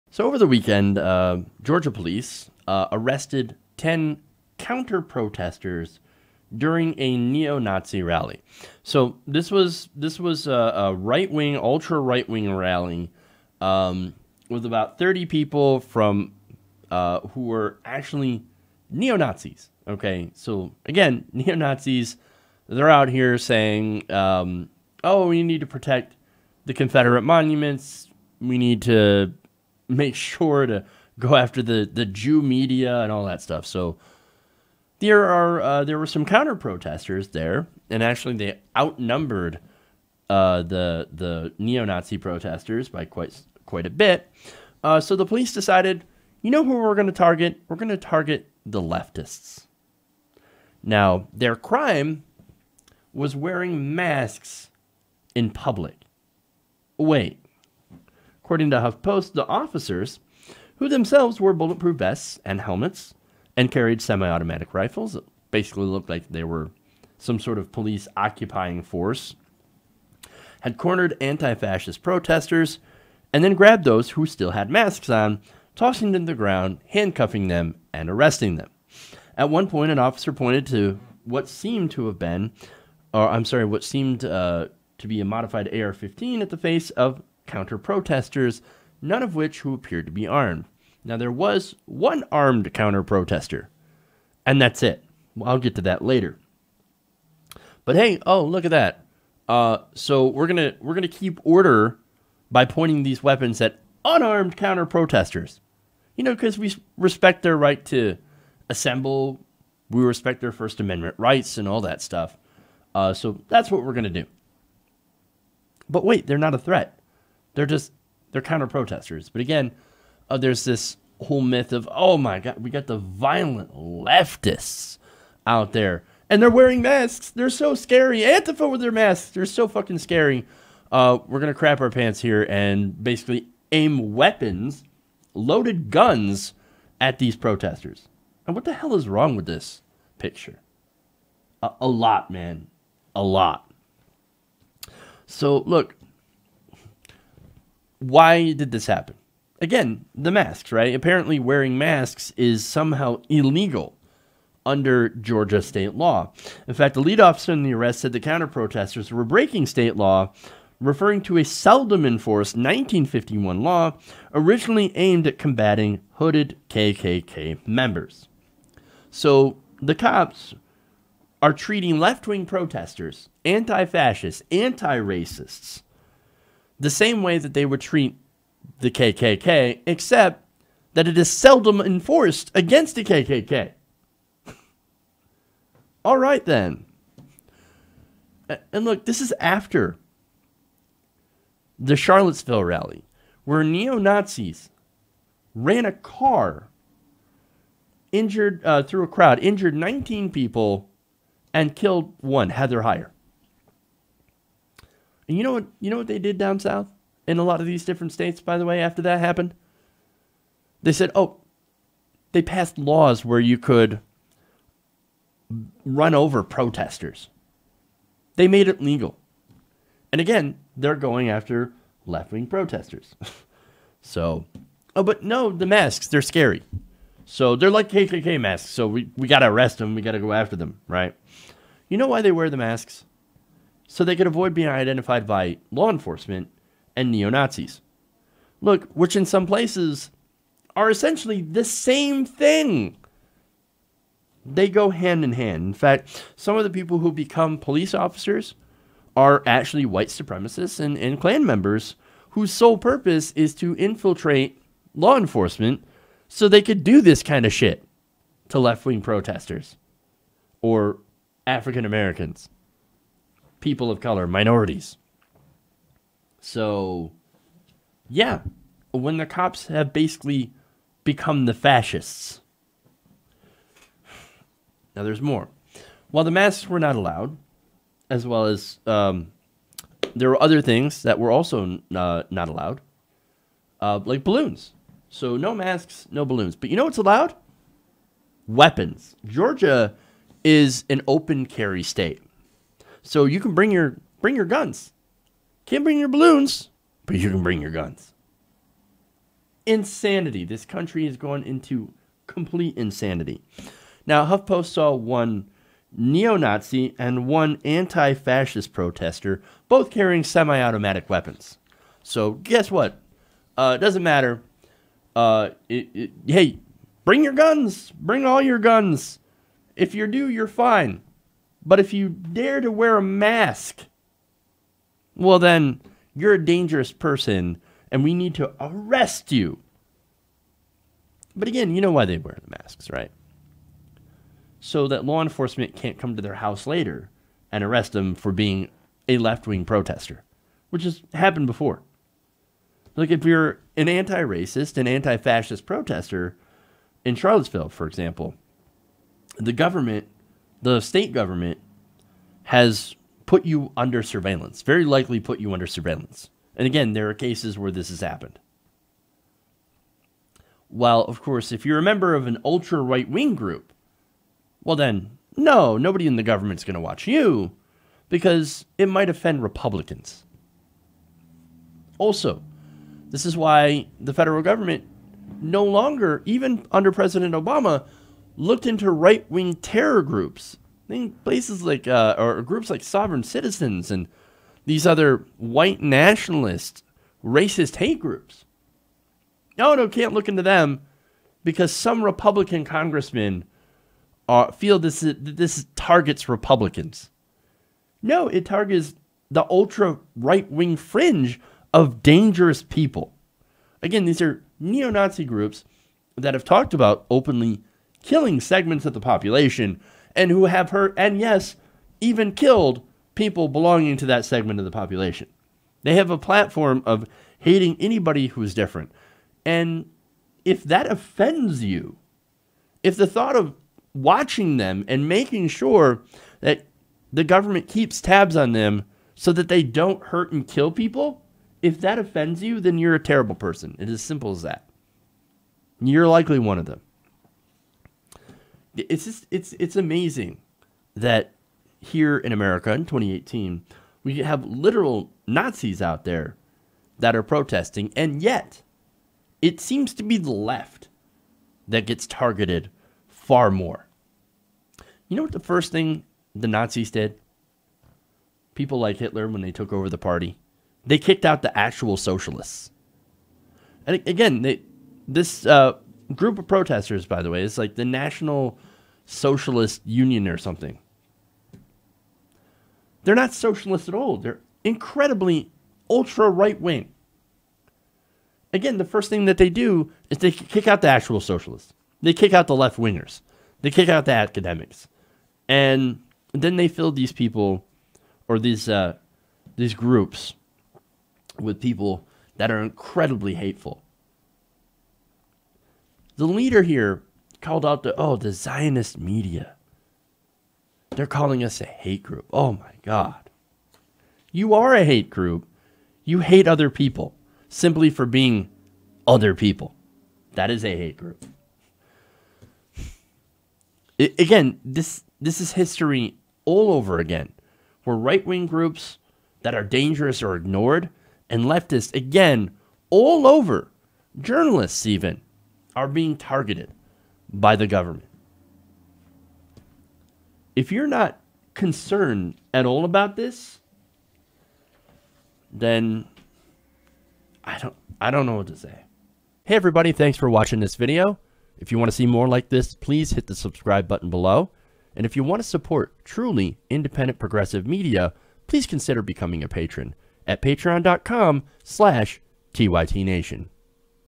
Heavily Armed Police Target Counter-Protestors At Neo-Nazi Rally
"A heavily militarized police force of some 400 officers aggressively patrolled a small neo-Nazi rally in this city 40 miles southwest of Atlanta on Saturday and arrested about 10 counterprotesters, many for the crime of wearing a mask.

Police officers arrived before the rally began and approached a group of about 50 anti-fascist protesters. They demanded the protesters remove their masks or face arrest. The officers — who wore bulletproof vests and helmets, and carried semi-automatic rifles — cornered the anti-fascist protesters, then grabbed those who were still masked, tossing them to the ground and handcuffing them."